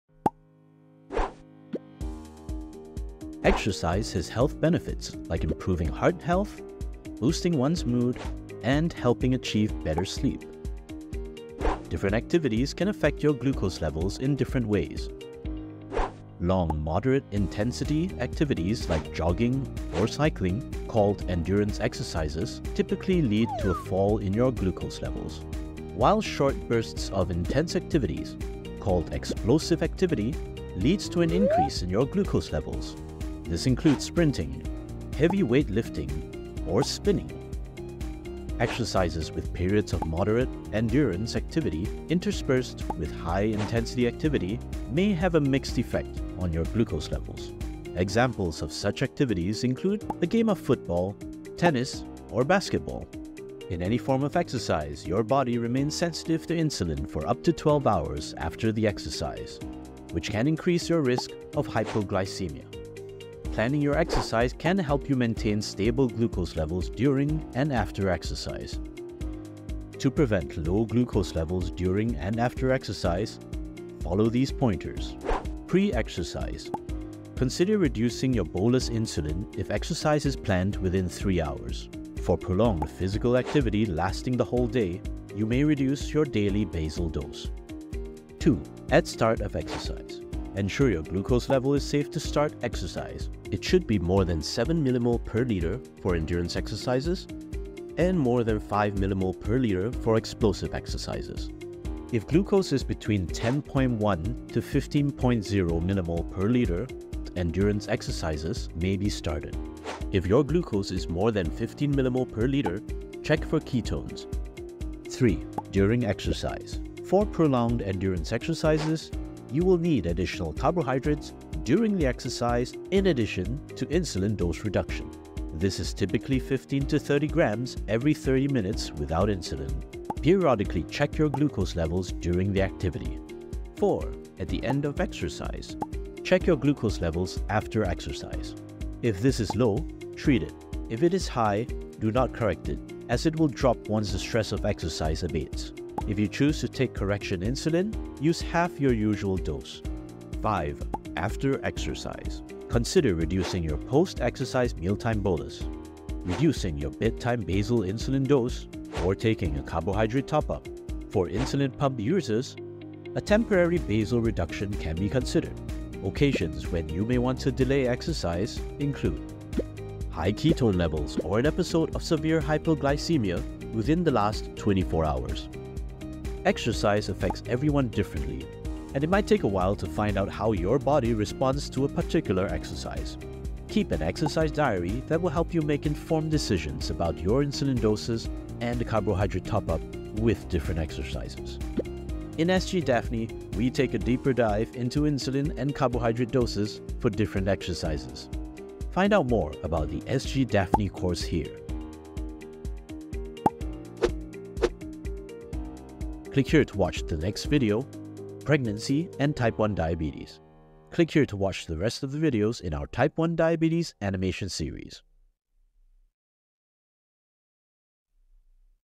Exercise and Type 1 Diabetes | Understanding Type 1 Diabetes
Exercise is important for physical and mental well-being. Yet exercise in Type 1 diabetes often results in challenges with high and low glucose levels during or after activity. Here we discuss the concepts of glucose changes in exercise and provide tips on how to manage this with insulin dose considerations and carbohydrate consumption for safe exercise.

This video is part of a patient education initiative supported by Medtronic.